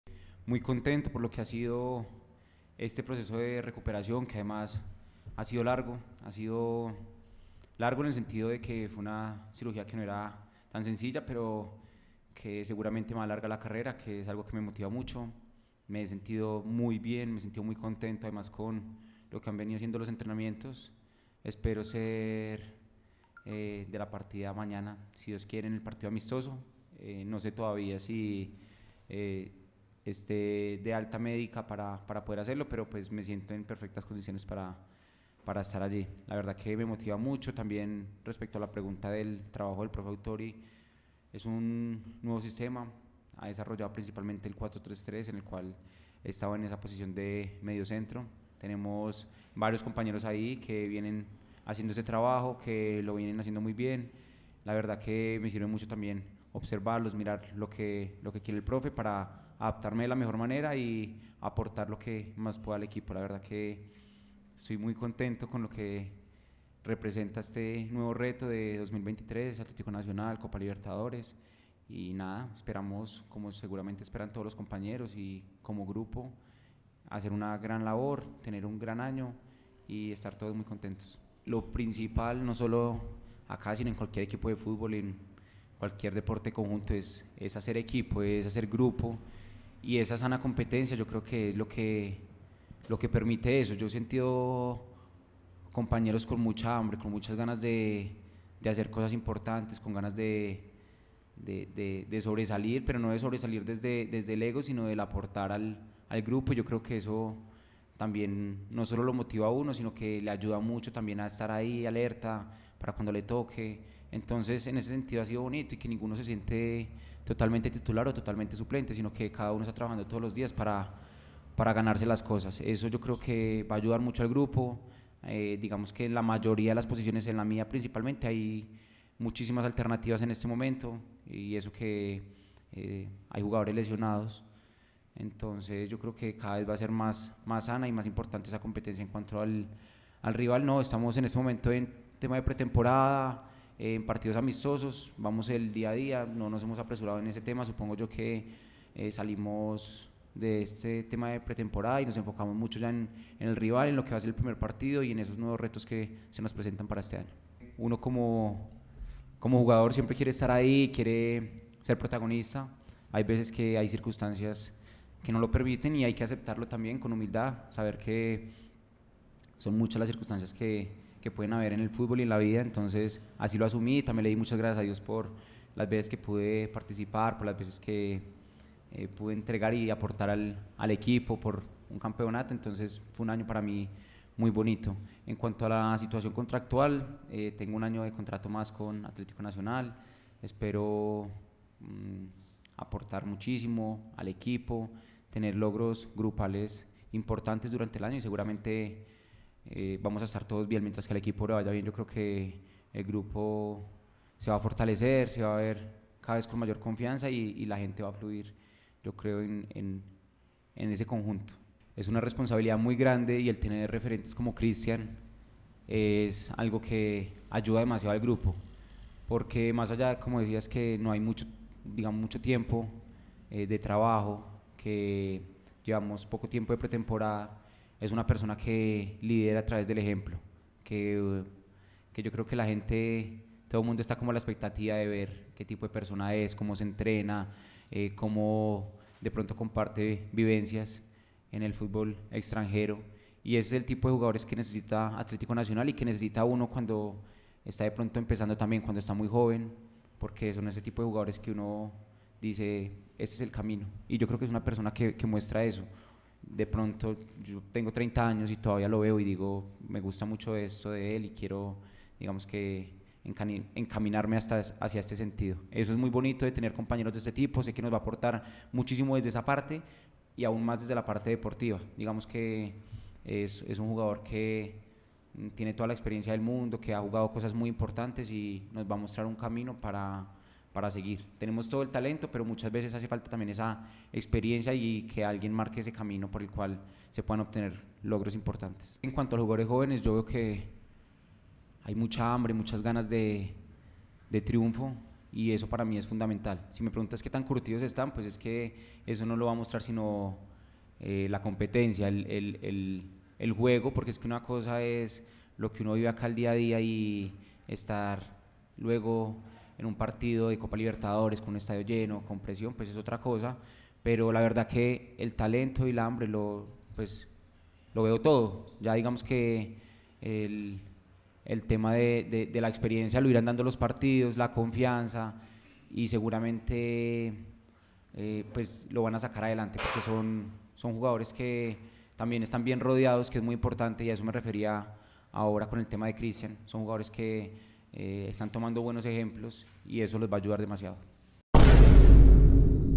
Noticias Atlético Nacional: Jhon Duque ELOGIA EL FICHAJE de Cristian Zapata ⚽️
Conferencia de prensa en atleticonacional

Jhon Duque habla de su regreso a las canchas, de Cristian Zapata y de cómo ve a los juveniles del equipo Verdolaga